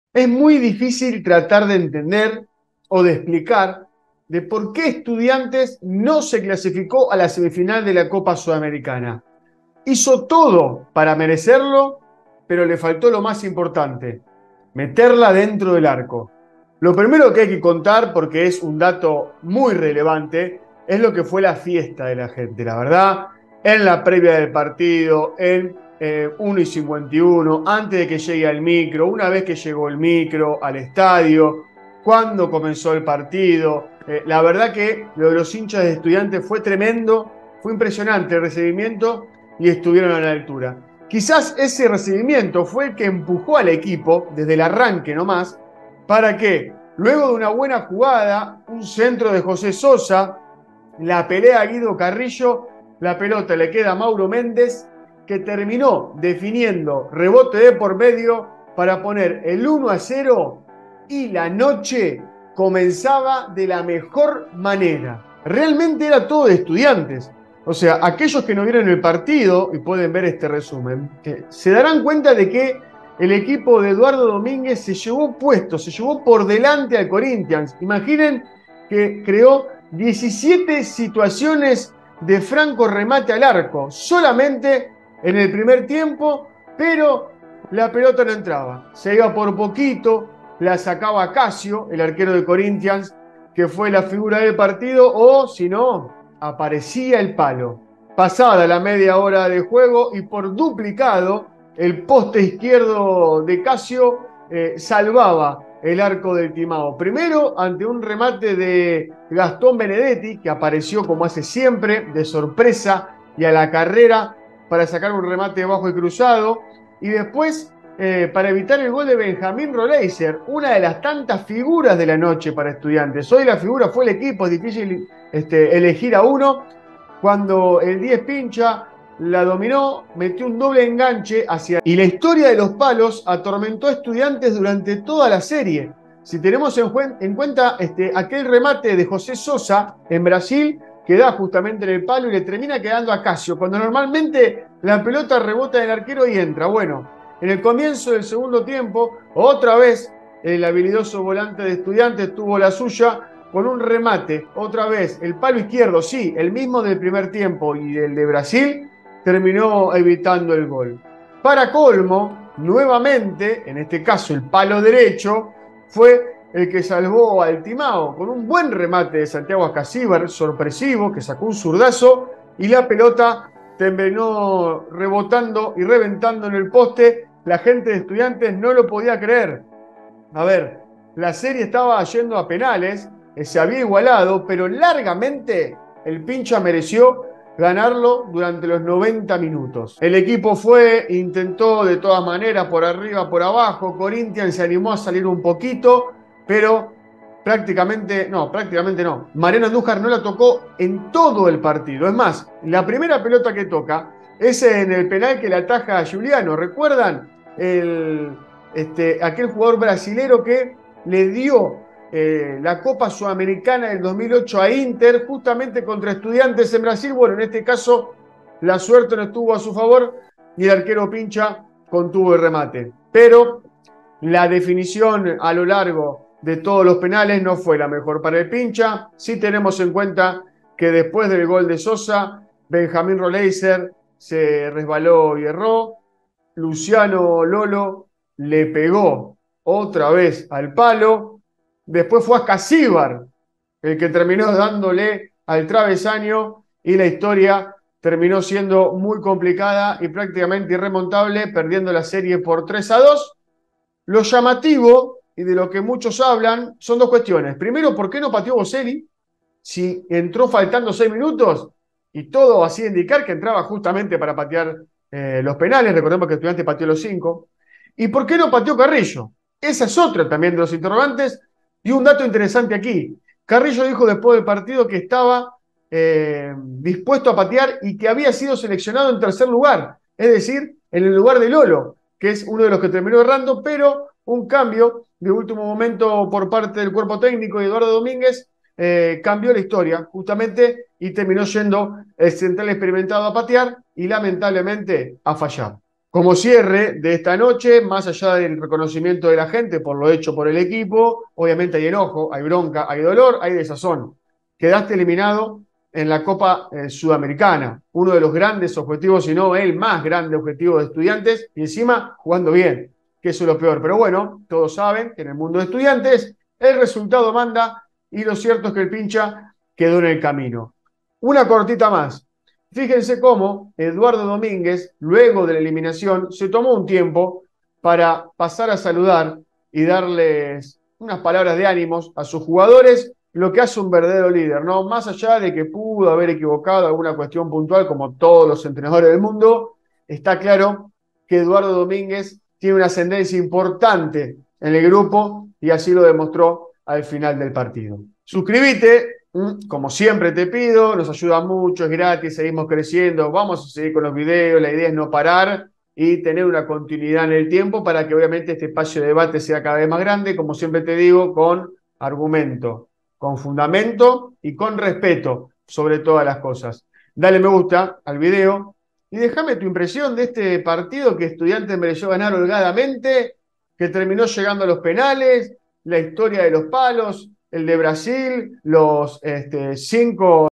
🦁 ESTUDIANTES 1 (2) vs. CORINTHIANS 1 (3): un palazo a la ilusión 😤
Estudiantes jugó quizás el mejor partido de la era Eduardo Domínguez, pasó por encima al Corinthians, pero en los penales se quedó sin nada. Los palos en La Plata y en Brasil conspiraron contra su suerte, y terminó eliminado de la Copa Sudamericana y con las manos vacías.